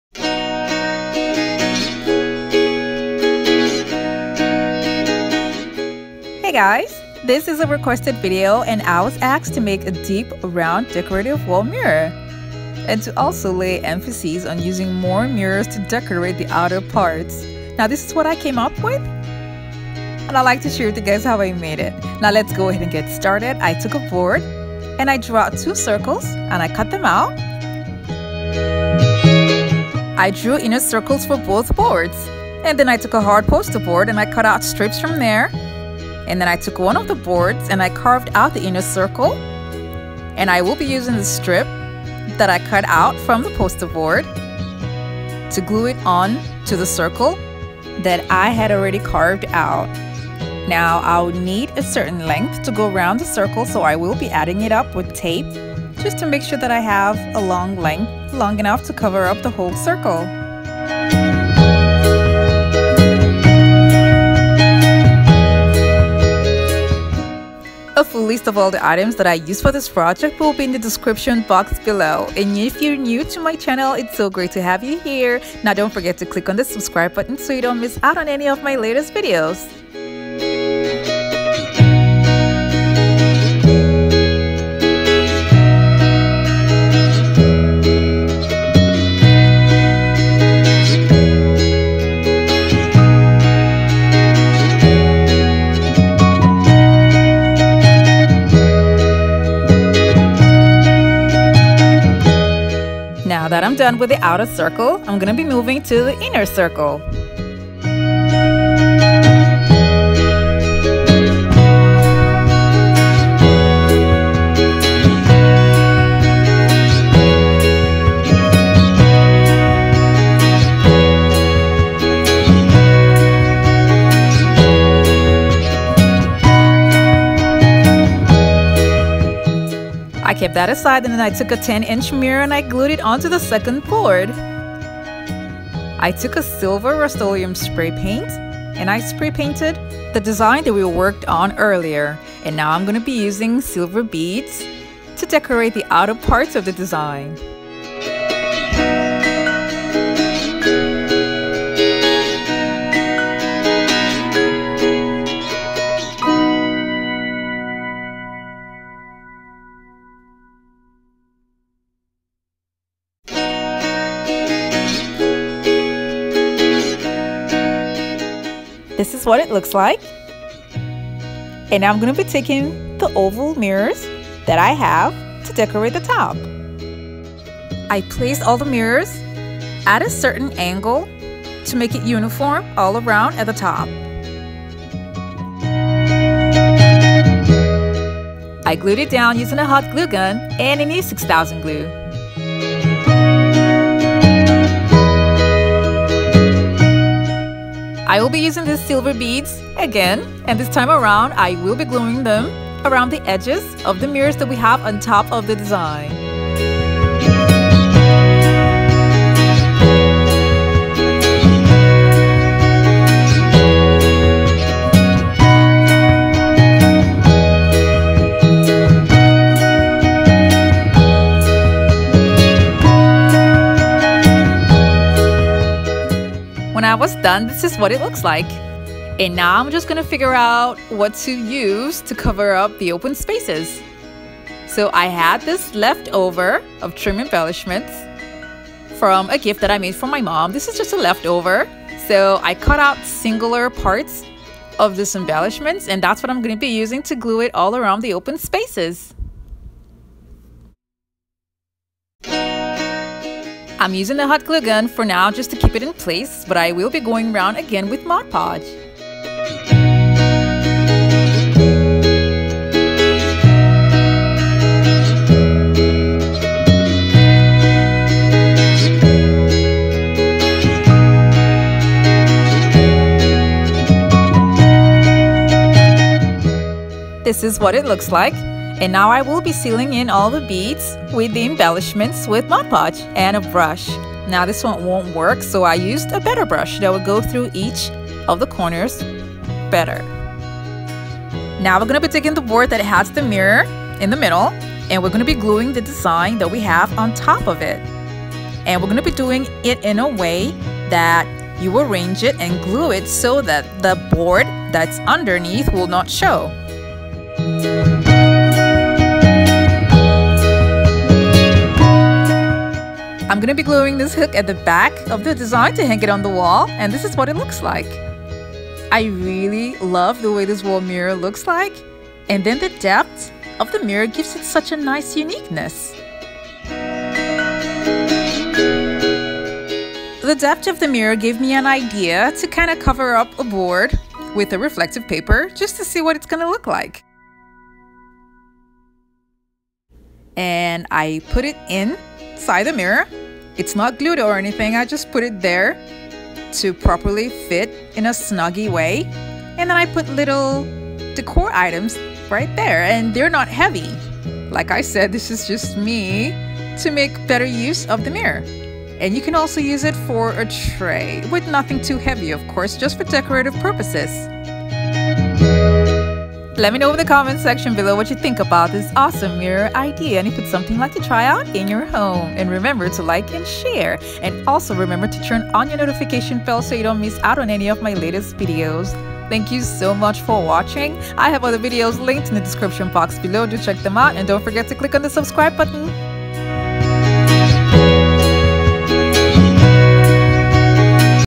Diy Deep Round Decorative Wall Mirror| Wall Decorating Ideas!
This Diy Round Mirror Design has a unique depth that adds to the Wall Decor in the Home. It is an Inexpensive Way to add Style to your Walls at Home while on a Budget! It is an Outstanding Mirror Decor while on a Budget using Regular Items! It is a Unique, Quick and Easy Mirror Decorating idea using Boards, beads and oval Mirrors to give it a Glam Look. It is a Wall Decorating idea that is eye Catching and it is so Easy to make.

It is a Budget Friendly and a Modern Home Decor That adds style and Uniqueness to the Home.

Here are the items used:
Oval mirrors:
Silver Beads
Trifold board
Poster board
Silver spray paint
Mod podge
E6000 glue
Hot glue gun and glue sticks